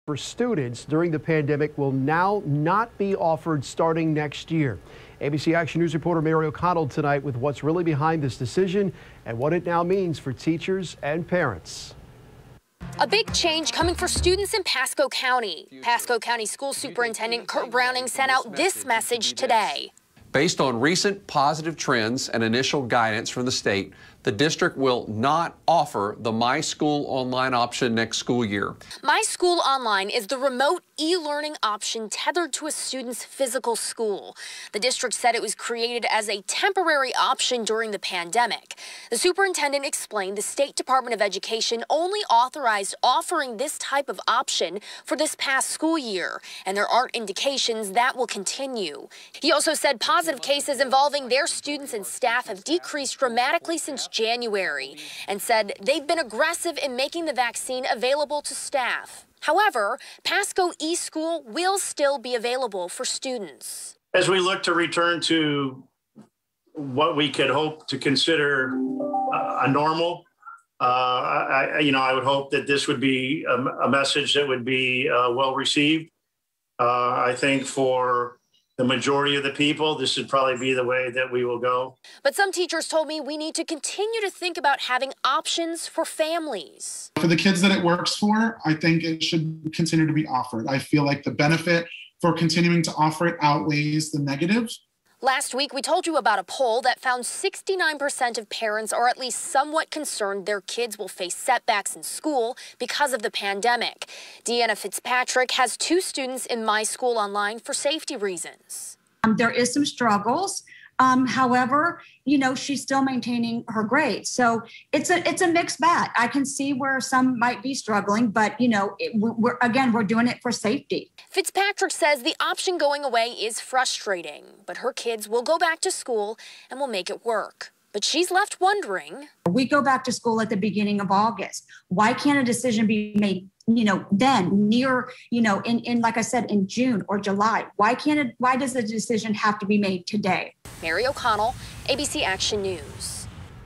Pasco Schools will not offer mySchool online learning for 2021-2022 school year, superintendent says
Pasco County Schools will not offer online learning next school year and expect normal operations to resume in the fall, Superintendent Kurt Browning announced in a Youtube video.